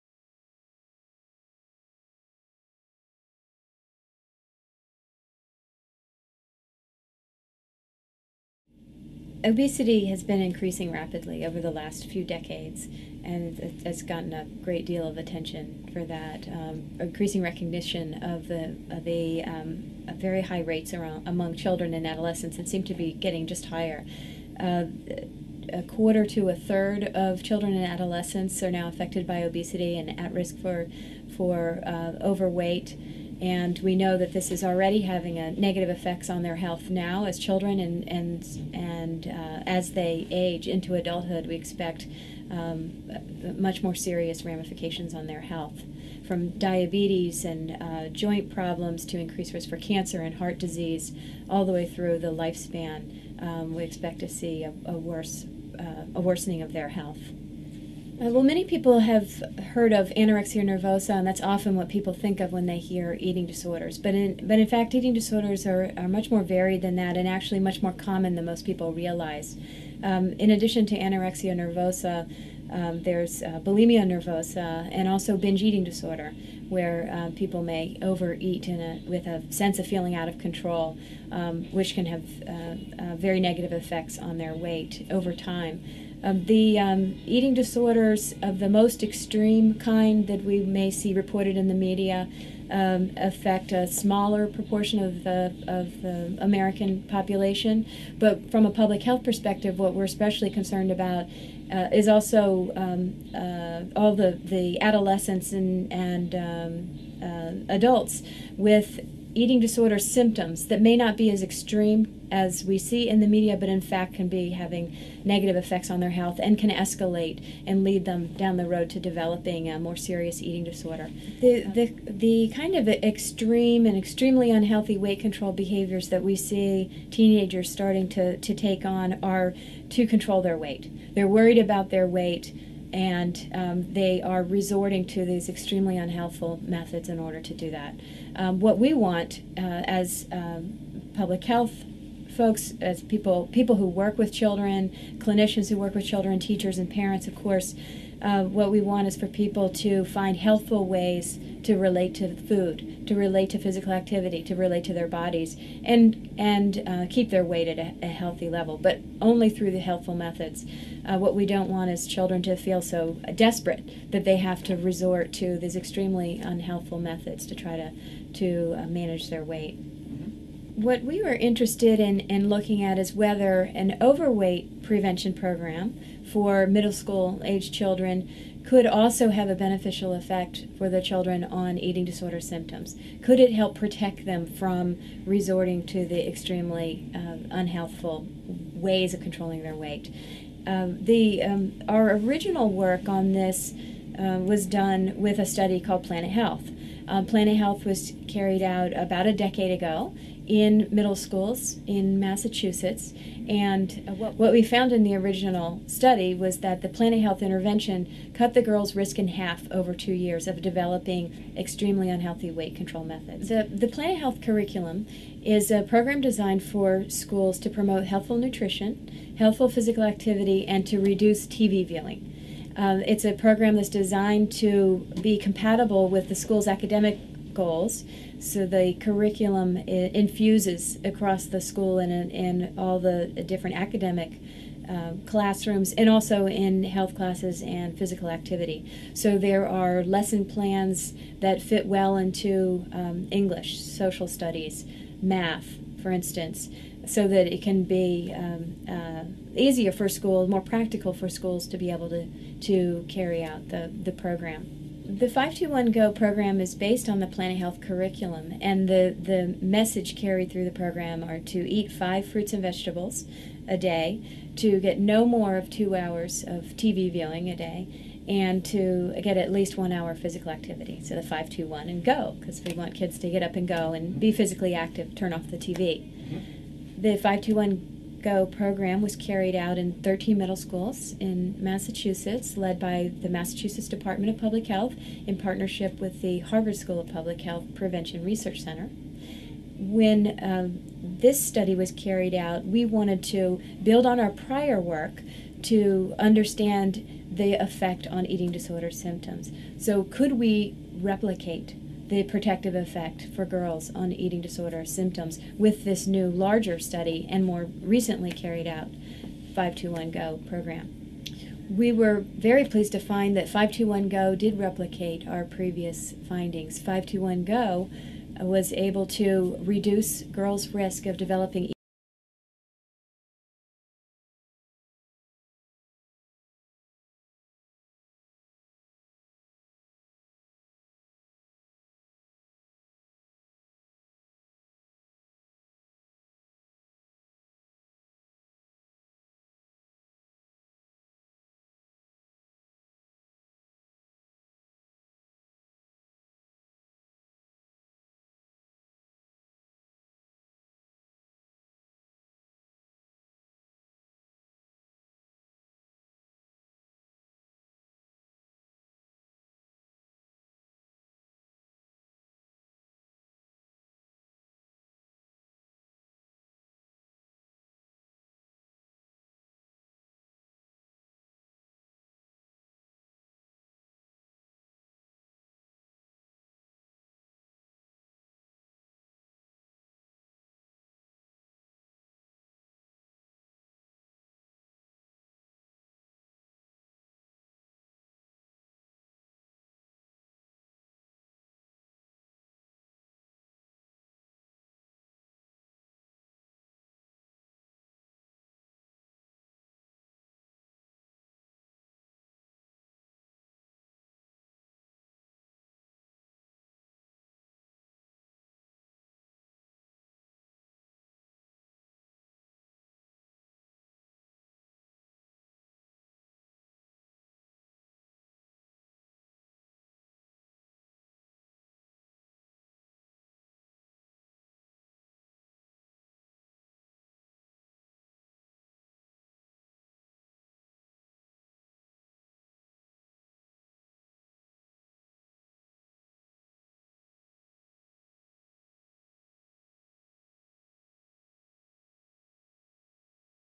S. Bryn Austin on Childhood Obesity
S. Bryn Austin, assistant professor at HSPH and a researcher at Children's Hospital in Boston, discusses a new study that showed that an obesity prevention program in middle schools may reduce the risk of eating disorders among girls.